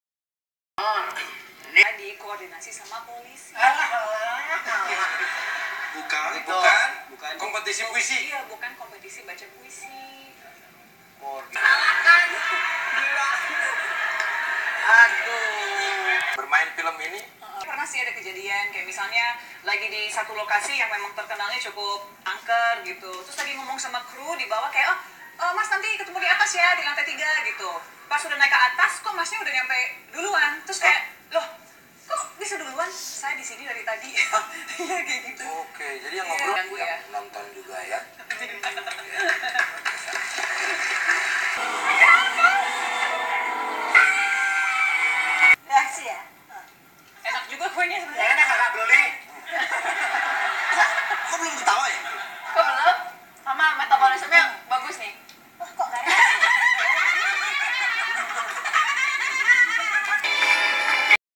Hannah Al Rashid
Hannah Al Rashid yang  wajahnya sangat cantik rupawan ini adalah aktris sekaligus aktivis yang pikirannya begitu menawan.